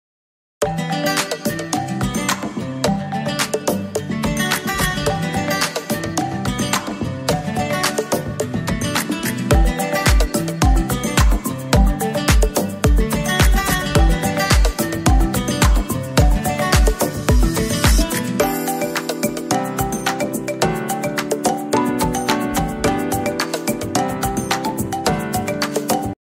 Best Shredding Moments - Satisfying ASMR Compilation
We collected the best & most satisfying shredding moments to you!